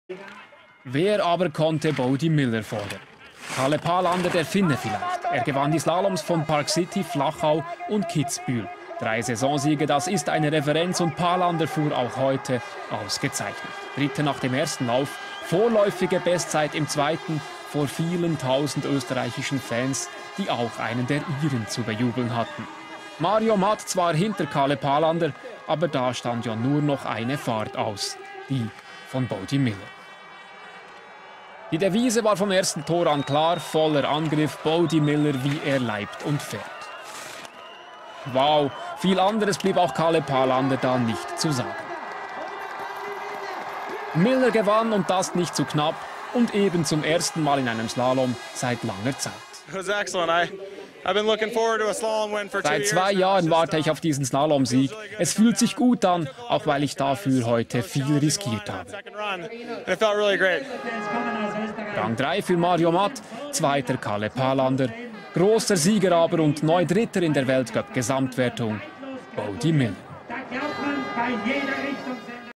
Bode Miller wins slalom (St. Anton 2004)
For the forth time in his career Bode Miller (USA) managed to win World Cup slalom race in St. Anton, Austria in 2003/04 season. Kalle Palander (FIN) was 2nd and Mario Matt (AUT) 3rd.